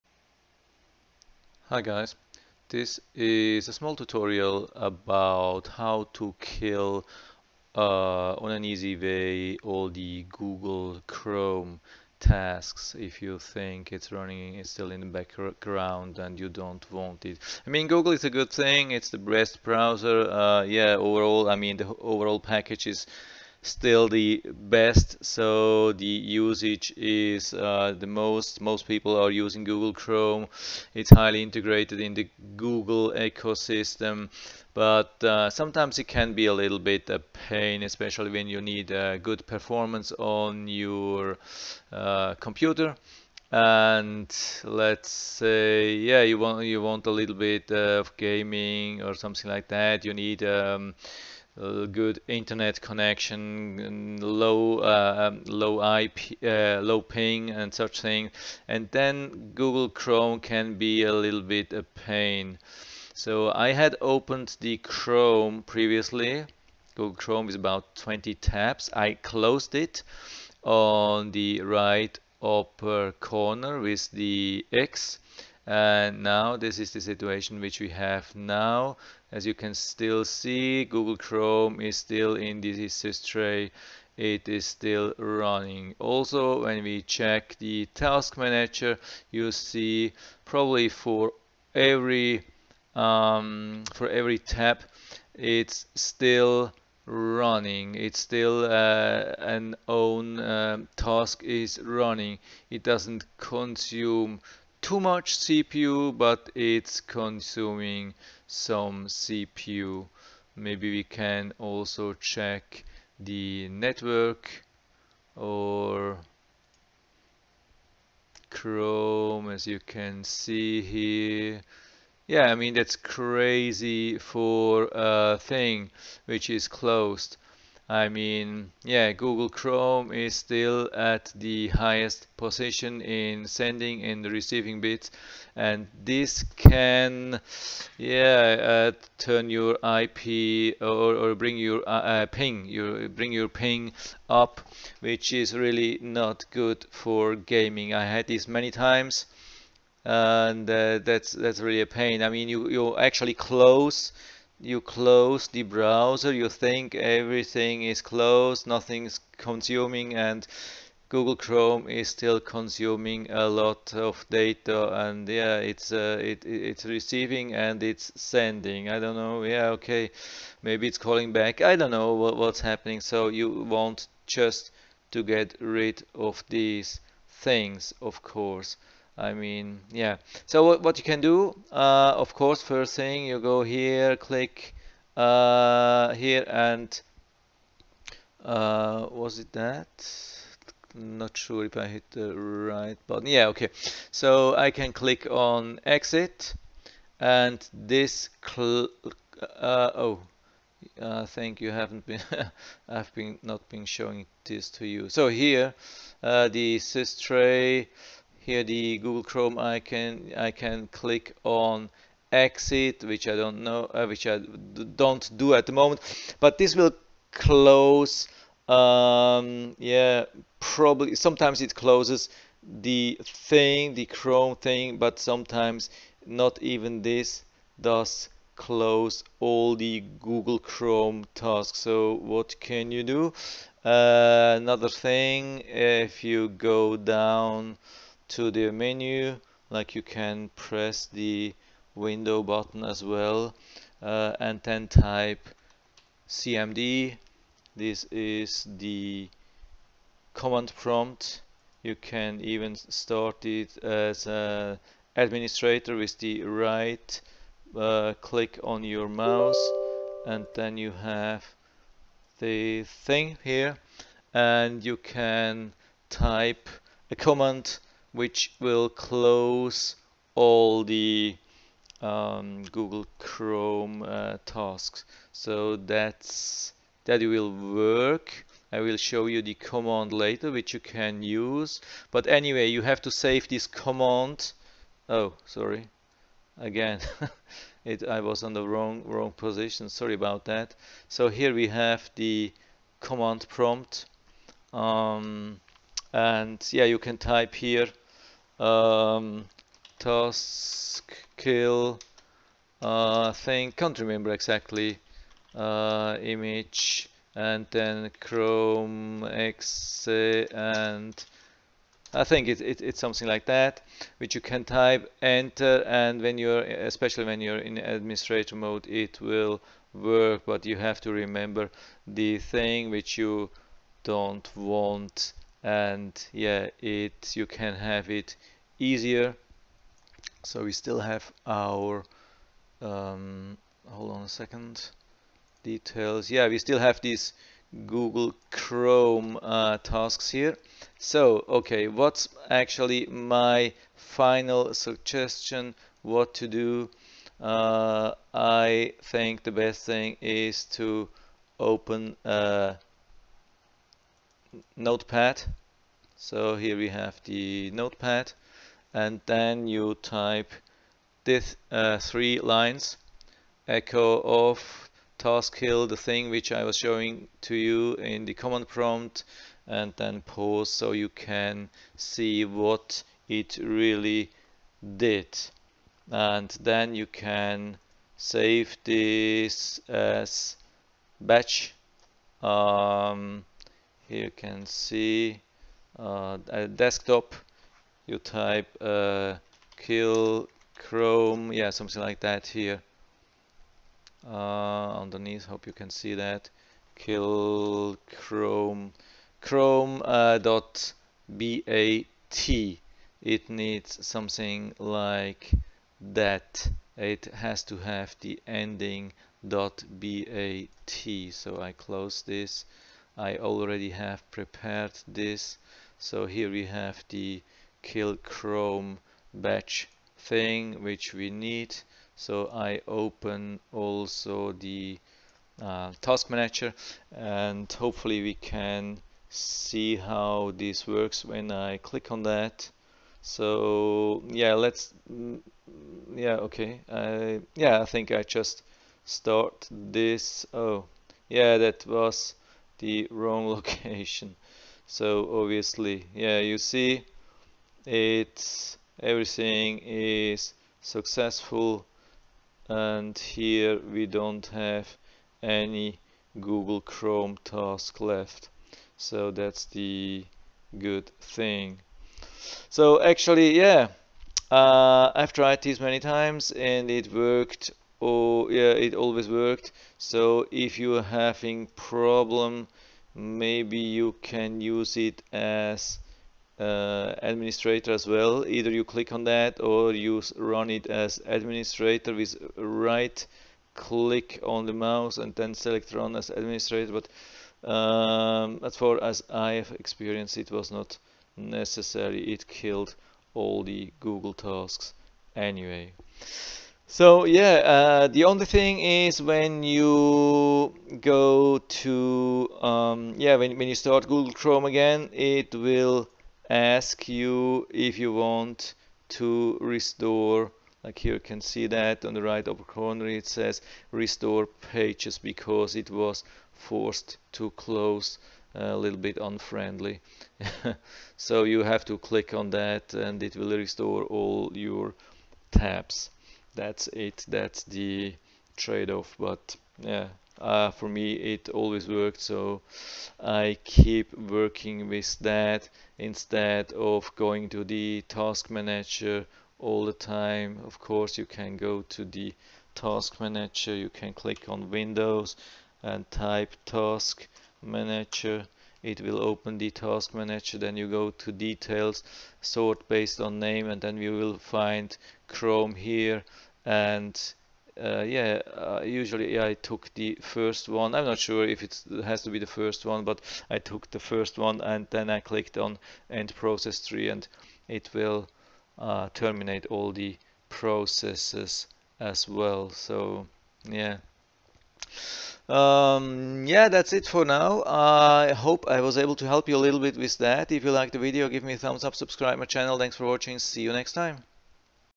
How To Kill Google Chrome Tasks Without Taskmanager To Get Lower Ping?
There's a much easier way than doing it with the taskmanager or by command-prompt. It can be fully automated. It can be helpful if you thing Google Chrome consumes too much CPU, RAM and bandwidth and especially rises your ping, which is not good for gaming.

Just watch the video. Hope this will help you.

BTW: this is the code, which you need for the "kill_chrome.bat" file:
@ECHO OFF
taskkill /f /im chrome.exe  /t
PAUSE
GLHF